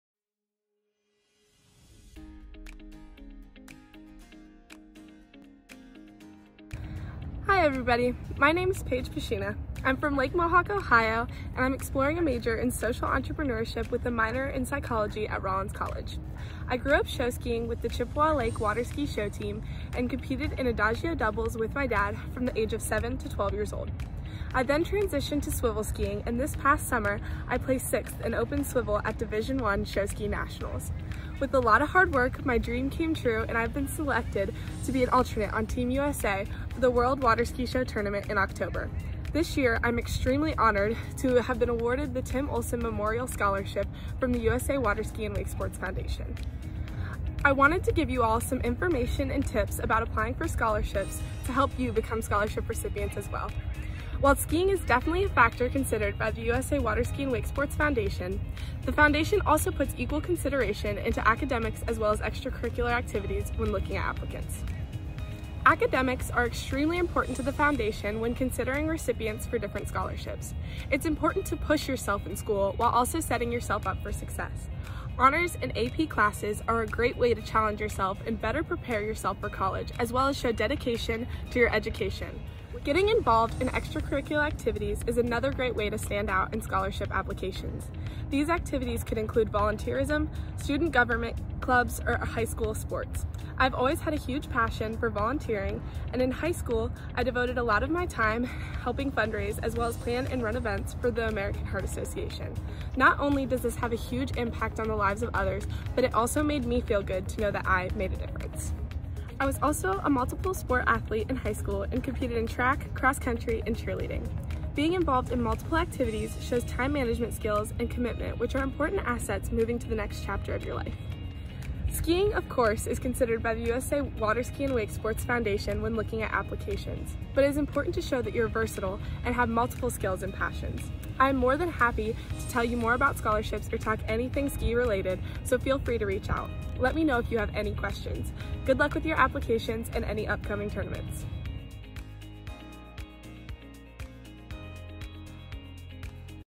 Paige Peshina's Scholarship Tips
2021 Tim Olson Memorial Scholarship Winner Paige Peshina explains how she prepared for scholarship success with water skiing, academics and extracurricular activities.